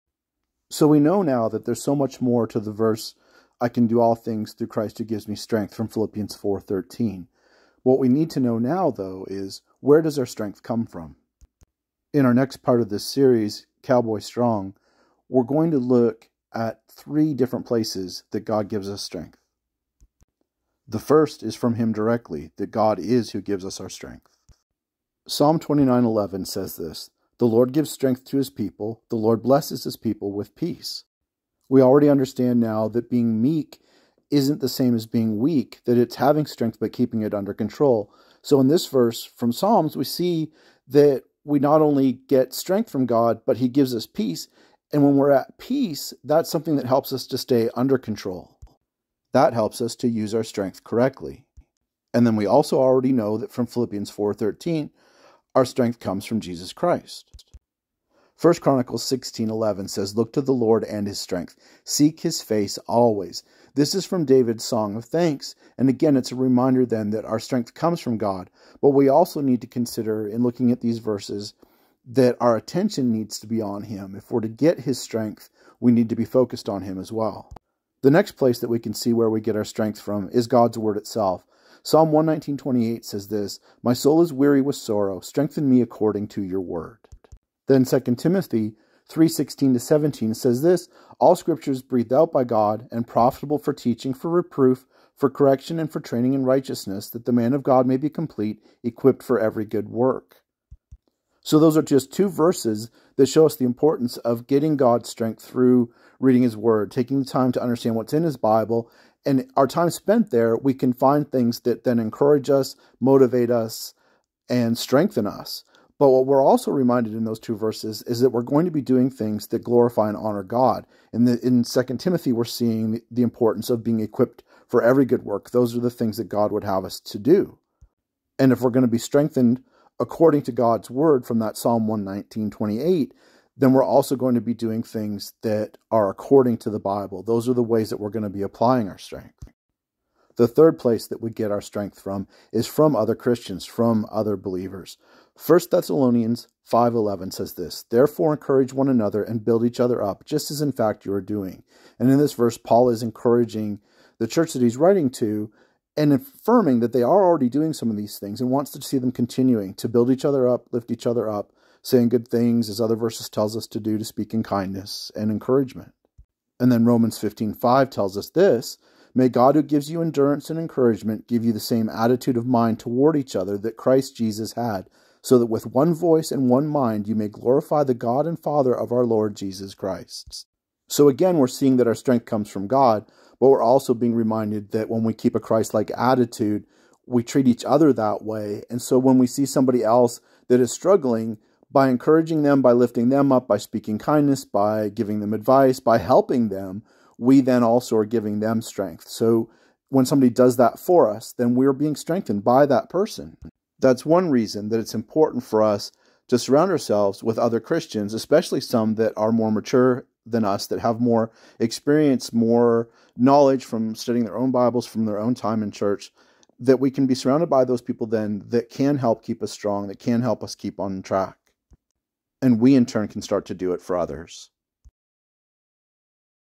Cowboy Strong Part 4: where do we get our strength
Part 4 in a series about what strength looks like in the Bible and how it differs and compares to being cowboy tough
CowboysOfTheCross.com.